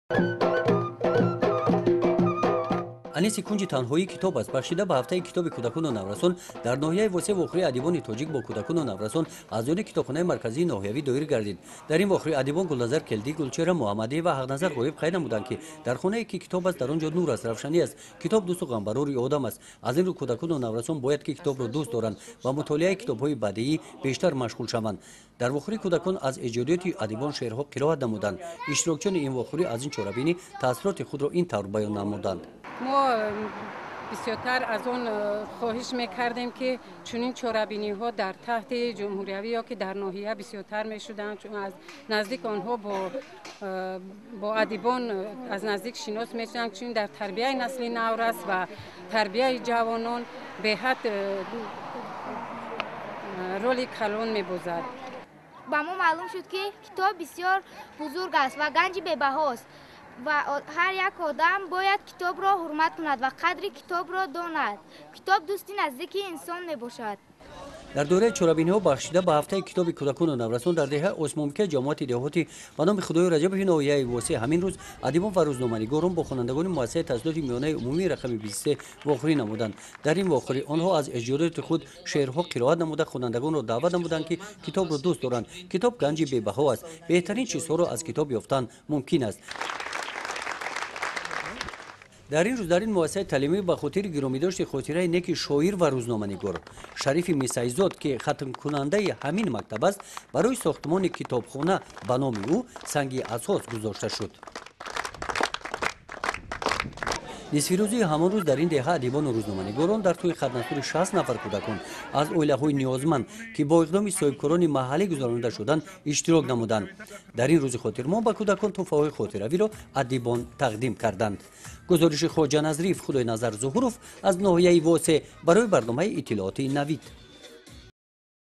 Хафтаи китоб - Восеъ
Дар доираи чорабинихо бахшида ба «Хафтаи китоби кудакону наврасон» дар нохияи Восеъ вохурии адибон бо кудакону наврасону ин нохия доир гардид. Аз ин вохури наврасон таасуроти хуб бардоштанд.